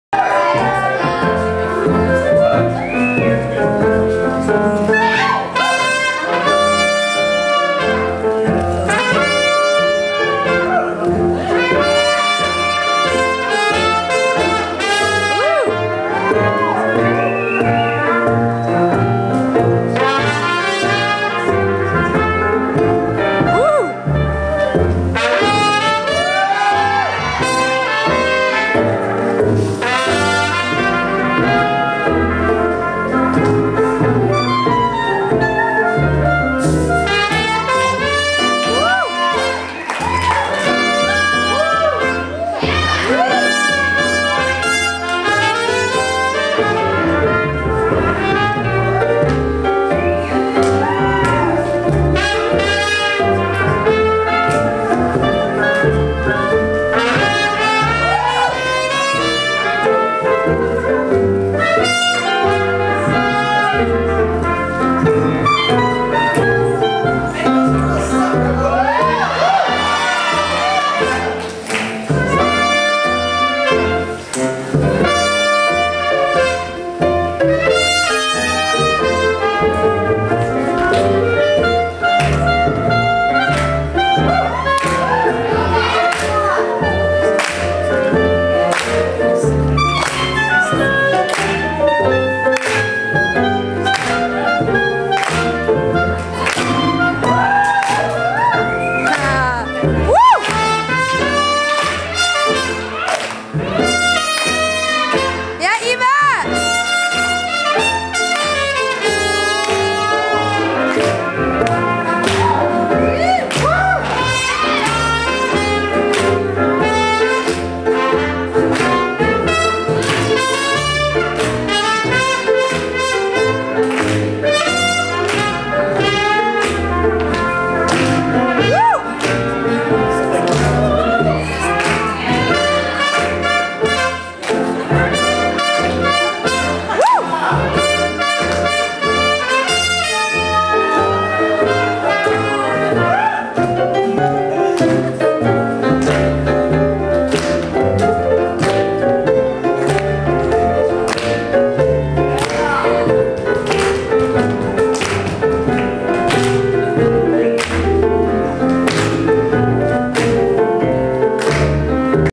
Killer Diller Weekend - Solo Charleston Finals Video 1 (of 4) - Slow
Finalists include (in order of appearance) Delila, Kevin Buster, Eva Robinson, Bobby Bonsey and Chris . Bobby placed 1st with Eva as runner up. Great job everyone!(will add more details soon)